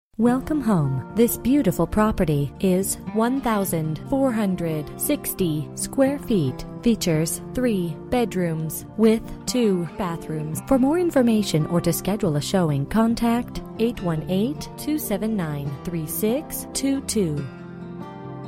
Residential for sale - 4489 MAPLEWOOD Place, Riverside, CA 92506
Beautiful mid-century home with a lots of potential. Three large bedrooms, 2 bathrooms with a spacious family room and kitchen. TLC needed

Bedrooms: 3
Bathrooms: 2 Bathrooms
Square Feet: 1460
Price: $350,000